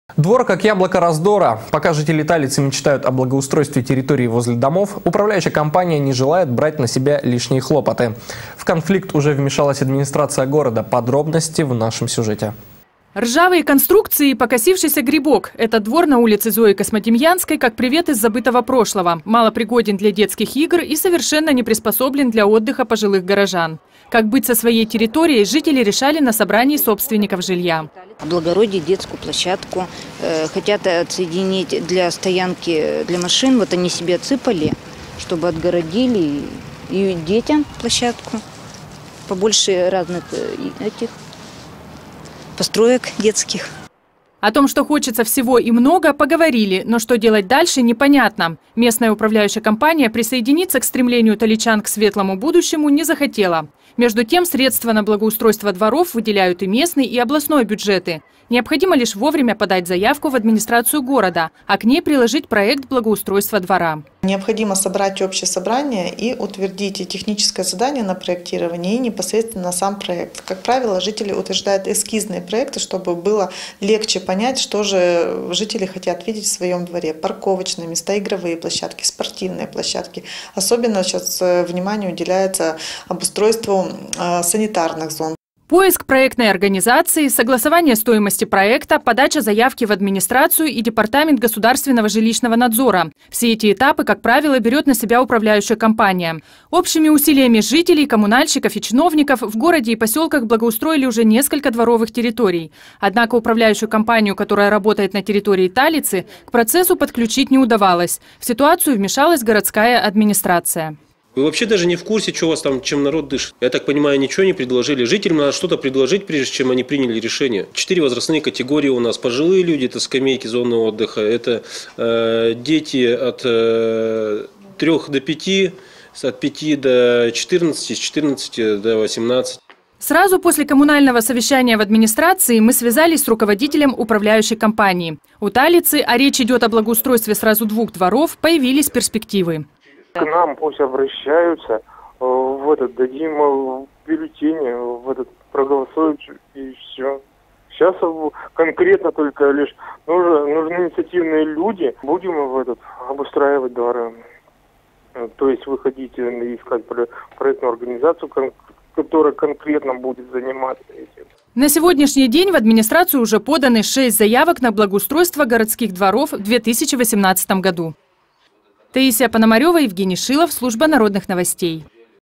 Двор как яблоко раздора
Пока жители Талицы мечтают о благоустройстве территории возле домов, управляющая компания не желает брать на себя лишние хлопоты.

Ржавые конструкции и покосившийся грибок - этот двор на улице Зои Космодемьянской, как привет из забытого прошлого. Малопригоден для детских игр и совершенно не приспособлен для отдыха пожилых горожан. Как быть со своей территорией - жители решали на собрании собственников жилья.

СВЕТЛАНА ЕВДОКИМОВА, ЖИТЕЛЬНИЦА ТАЛИЦЫ: « Облагородить детскую площадку. Хотят отсоединить для стоянки для машин, вот они себе отсыпали, чтобы отгородили, и детям площадку - побольше разных построек детских».

О том, что хочется всего и много, поговорили, но что делать дальше - непонятно. Местная управляющая компания присоединиться к стремлению таличан к светлому будущему, не захотела. Между тем, средства на благоустройство дворов выделяют и местный, и областной бюджеты. Необходимо лишь вовремя подать заявку в администрацию города, а к ней приложить проект благоустройства двора.

 МАРИНА ШОЛОХОВА, НАЧАЛЬНИК УПРАВЛЕНИЯ ЖКХ И СТРОИТЕЛЬСТВА: «Необходимо собрать собрание и утвердить техническое задание на проектирование и непосредственно сам проект - как правило, жители утверждают эскизные проекты, чтобы было легче понять, что же хотят жители увидеть в своем дворе: парковочные места, игровые площадки, спортивные площадки, особенно внимание уделяется обустройству санитарных зон».

Поиск проектной организации, согласование стоимости проекта, подача заявки в администрацию и Департамент государственного жилищного надзора - все эти этапы, как правило, берет на себя управляющая компания. Общими усилиями жителей, коммунальщиков и чиновников в городе и поселках благоустроили уже несколько дворовых территорий. Однако управляющую компанию, которая работает на территории Талицы, к процессу подключить не удавалось. В ситуацию вмешалась городская администрация.

АРТУР ГУЗАИРОВ, ЗАМЕСТИТЕЛЬ ГЛАВЫ АДМИНИСТРАЦИИ ПЕРВОУРАЛЬСКА: «Вы вообще даже не в курсе, чем народ у вас там дышит. Я так понимаю ничего не предложили. Жителям же надо что-то предложить, прежде чем они приняли решение. Четыре возрастные категории у нас: это пожилые люди - скамейки, зоны отдыха, это дети от 3 до 5, от 5 до 14, с 14 до 18».

Сразу после коммунального совещания в администрации, мы связались с руководителем управляющей компании. У Талицы, а речь идет о благоустройстве сразу двух дворов, появились перспективы.

ЕВГЕНИЙ ЛЕЩЕВ,  ДИРЕКТОР ЖК "МАГНИТКА": «К нам пусть обращаются, дадим бюллетени, проголосуем. Конкретно только нужны инициативные люди. Будем обустраивать дворы, то есть выходить искать проектную организацию, которая конкретно будет заниматься этим».

На сегодняшний день в администрацию уже поданы 6 заявок на благоустройство городских дворов в 2018 году.